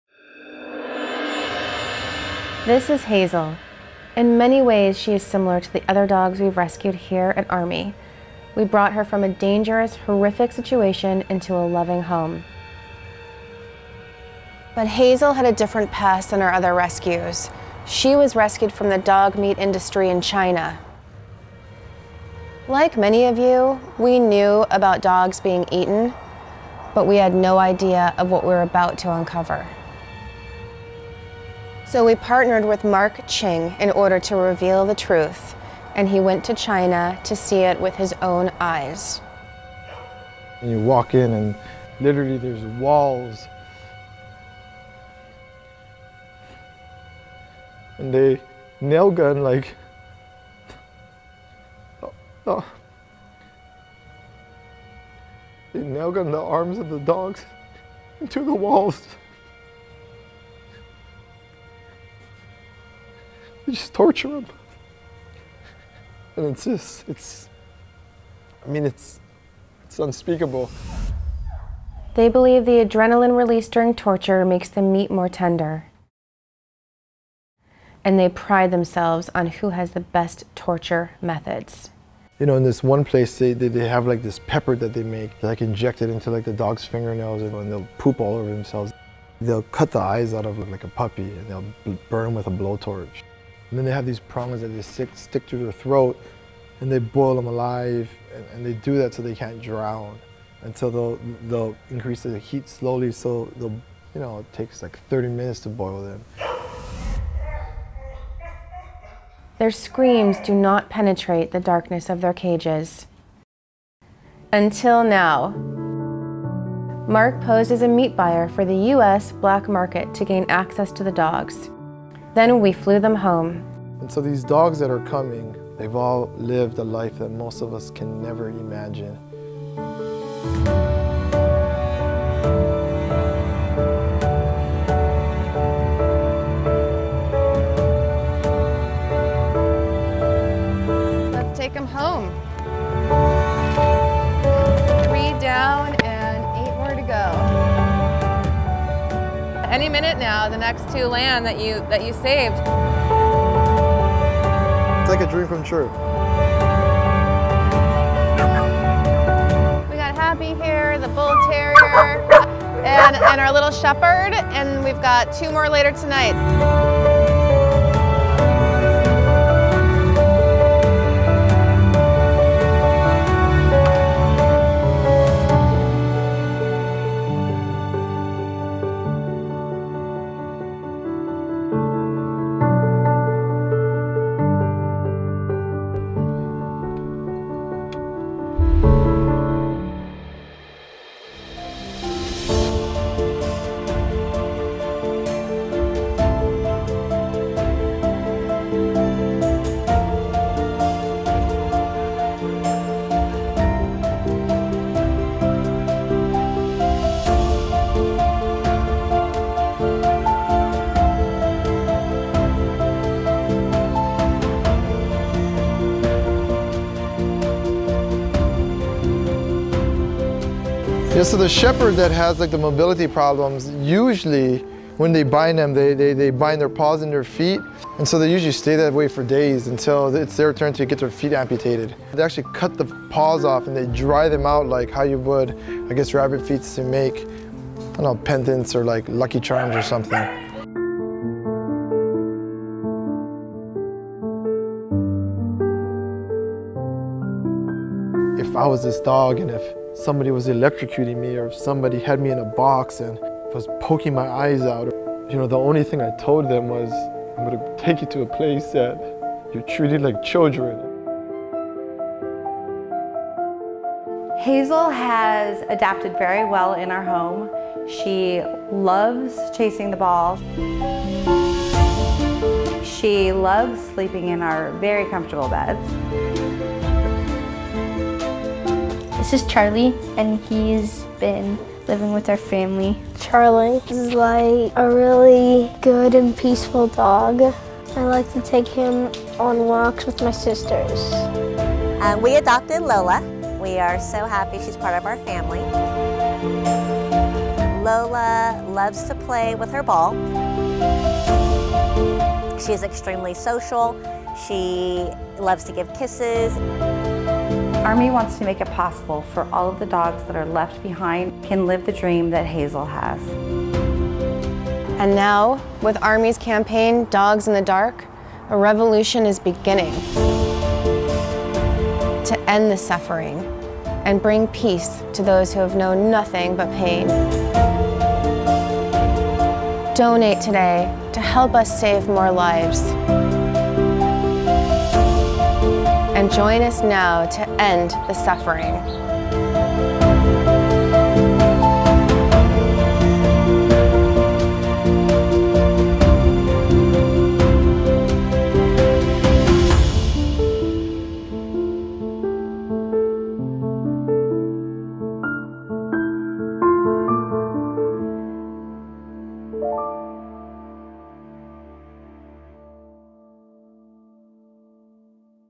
11 dogs rescued from dog meat
ARME helped rescue 11 dogs from the dog meat trade in China. Please watch this shocking and powerful footage.